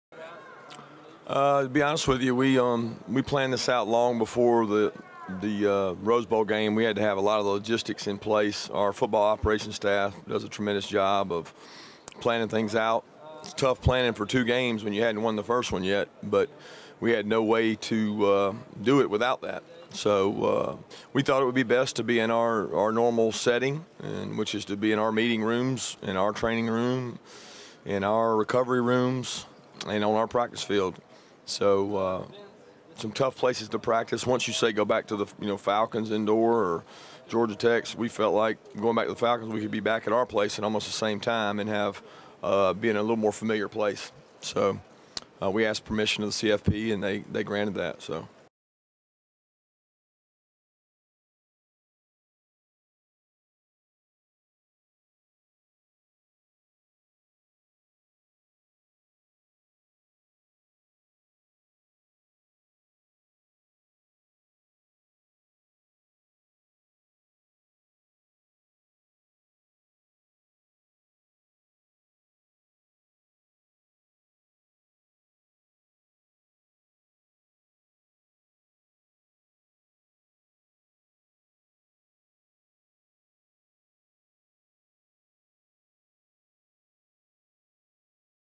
Georgia Head Coach Kirby Smart on the wisdom of practicing in Athens
UGA head coach Kirby Smart discusses practicing in Athens instead of Atlanta